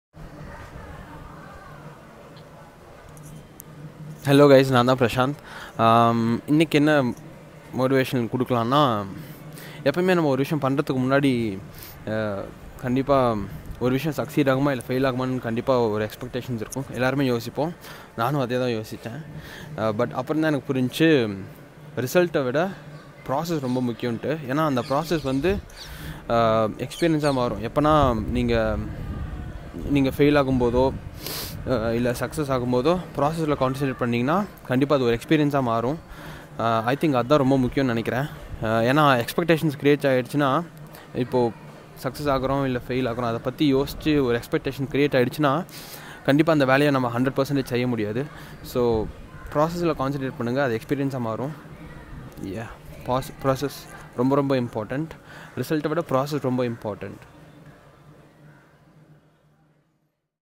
PROCESS is More Important | Expectation Kills | Monday Motivation | Prasanth MS
Process is more Important than the Result. PROCESS is Important than the RESULT.

Focus on the Process. It will turnout into Experience, even if U lose.

Expectation Kills. You can’t give 100%, If u think about the Result.